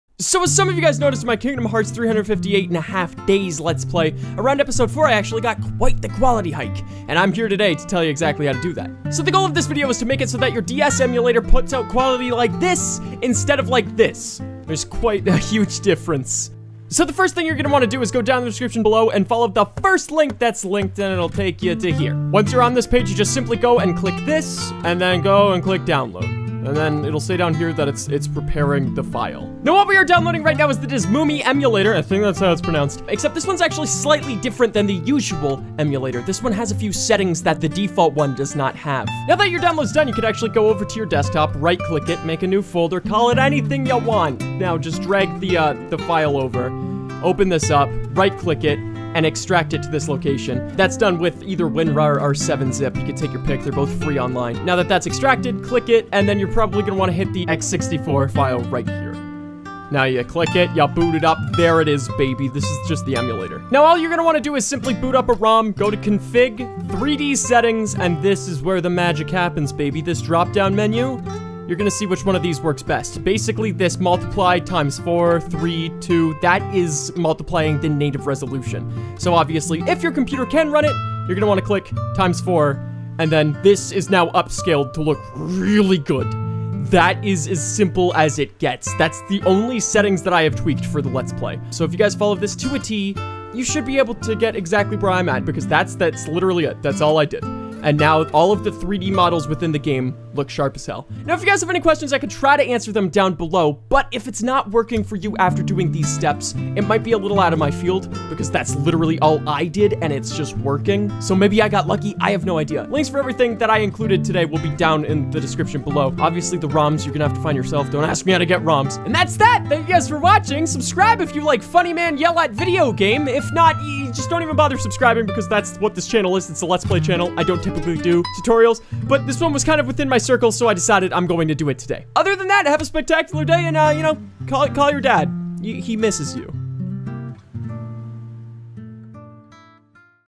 How To Make Nintendo DS Emulation LOOK BETTER (DeSmuME Tutorial)
Here's everything required:

The emulator provided is slightly different that the one used in the vide, however, the process and result is largely the same.